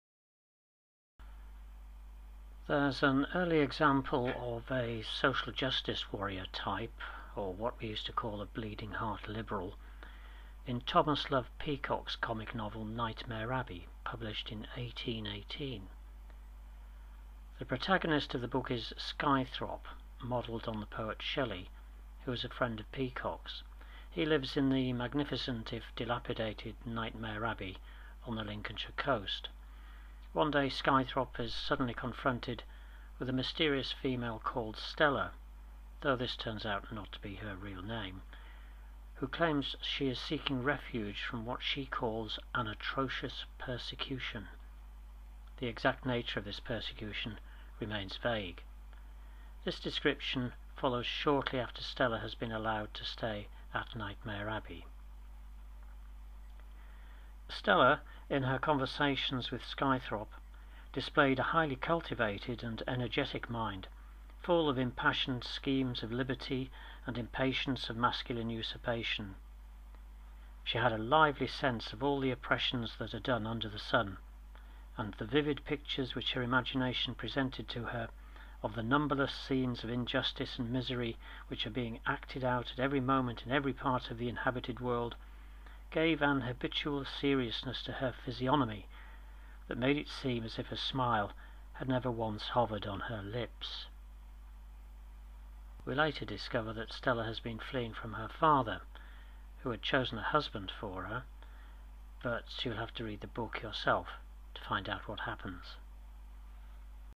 Stella, the social justice warrior; in Nightmare Abbey by Thomas Love Peacock (1818)
Thomas Love Peacock, in his 1818 comic novel, Nightmare Abbey, presents us with Stella, an early version of the social justice warrior.

If you enjoyed this or other videos on my channel please consider showing your appreciation by making a donation, however small, via PayPal